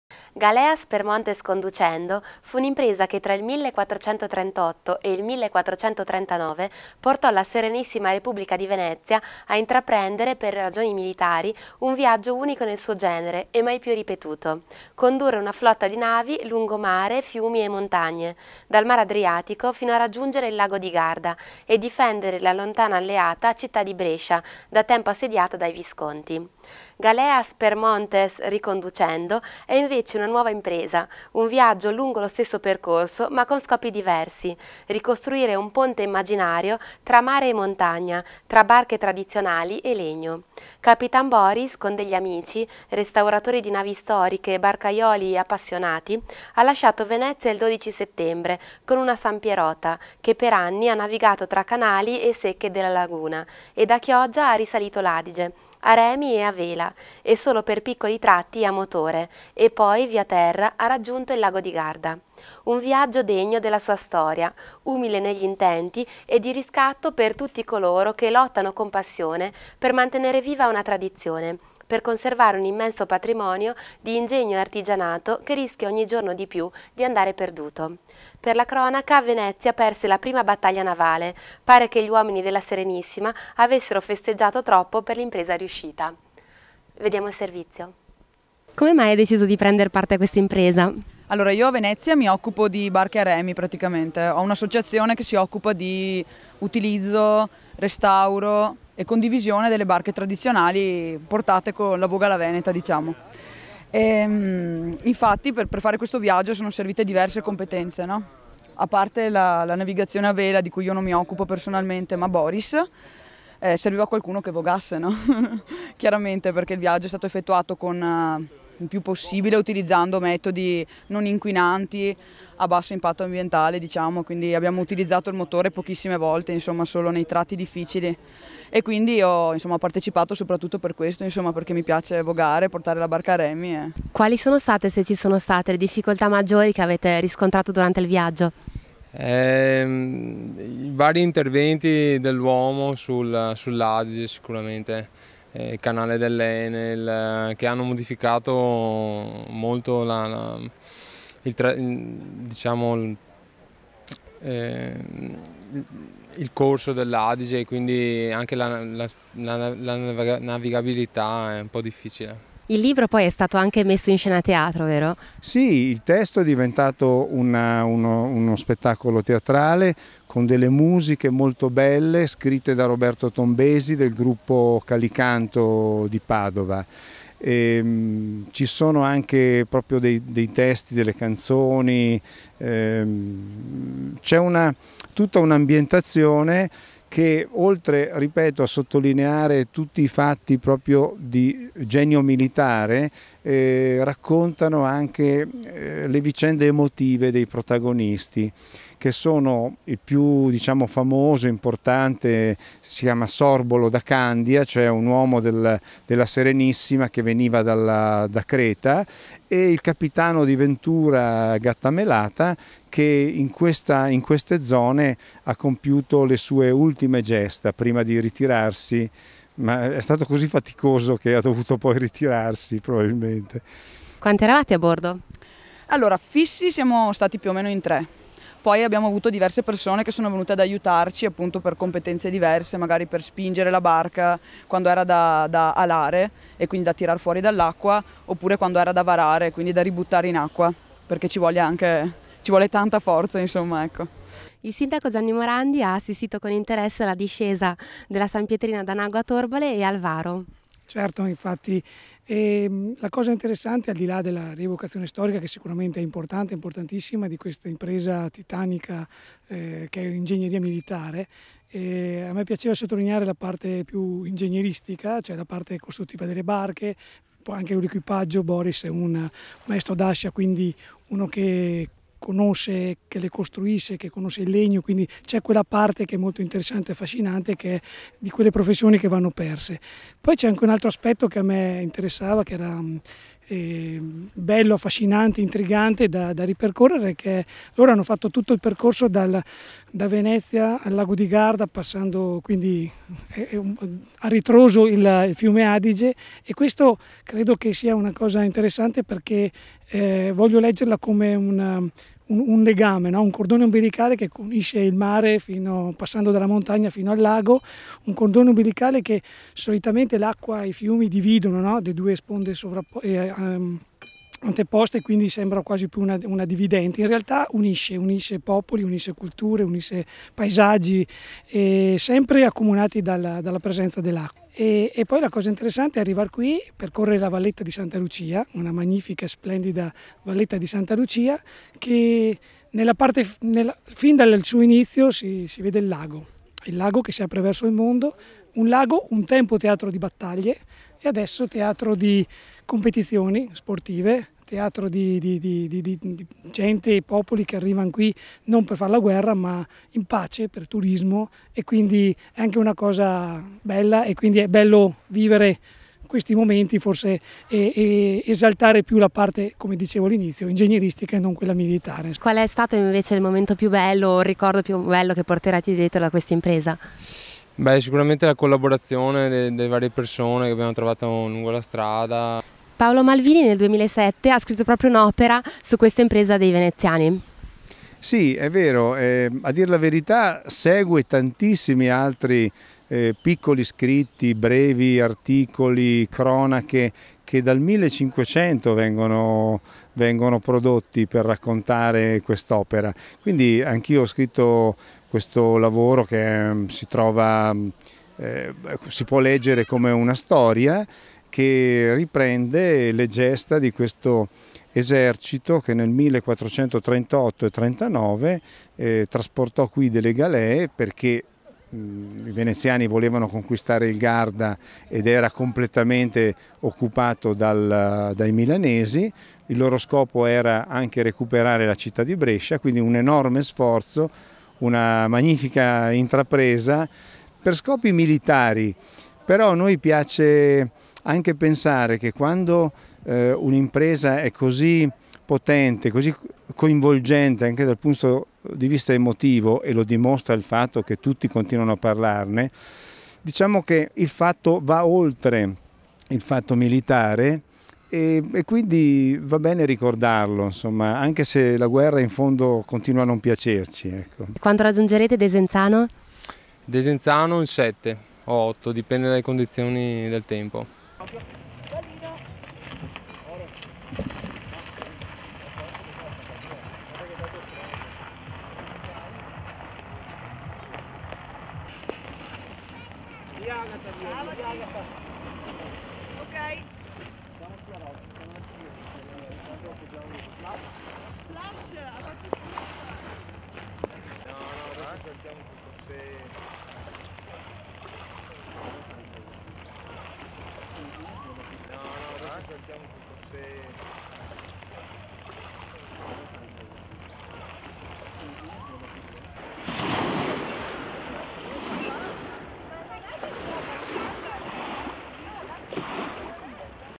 Galeas per montes Ri-Conducendo
Servizio del telegiornale del 4 ottobre 2015.

C'è chi ha portato un nave d'acqua salata nel Lago di Garda. Riuscirà a galleggiare ancora?